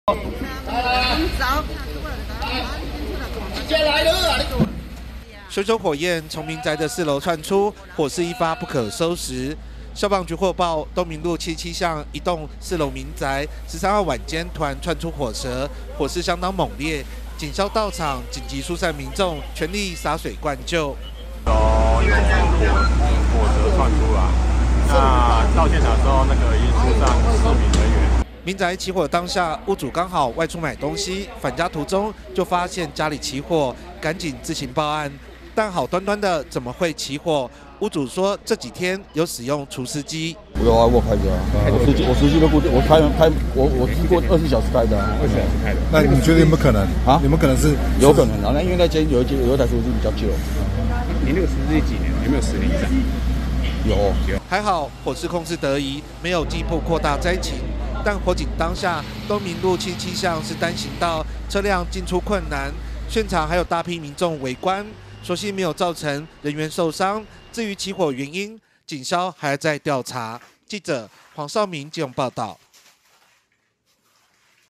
火舌 ｜火警｜東明路77巷民宅竄火舌 屋主外出躲過一劫
(中嘉吉隆新聞 記者黃紹銘 /基隆報導) 基隆東明路77巷晚間發生民宅大火，屋主短暫外出返家時，就發現家裡陷入火海，所幸警方緊急疏散民眾，沒有造成人員傷亡，屋主表示，連續幾天有使用除濕機，懷疑是電線走火，至於真的起火原因仍有待調查。
熊熊火焰，從民宅四樓竄出，火勢一發不可收拾。
消防局獲報，東明路77巷一棟民宅四樓，十三日晚間突然竄出火舌，火勢猛烈，警消到場，緊急速散民眾，權力灑水灌救。
民宅起火當下，屋主剛好外出買東西，返家途中，就察覺家裡起火，趕緊自行報案，但好端端怎麼會起火，屋主說，這幾天有使用除濕機。
還好火勢控制得宜，沒有進一步擴大災情，但火警當下，東明路77巷是單行道，車輛難以進出，現場有大批民眾圍觀，所幸沒有造成人員受傷，至於起火原因，消防火調人員已經展開調查，不排除是電線走火引發火勢。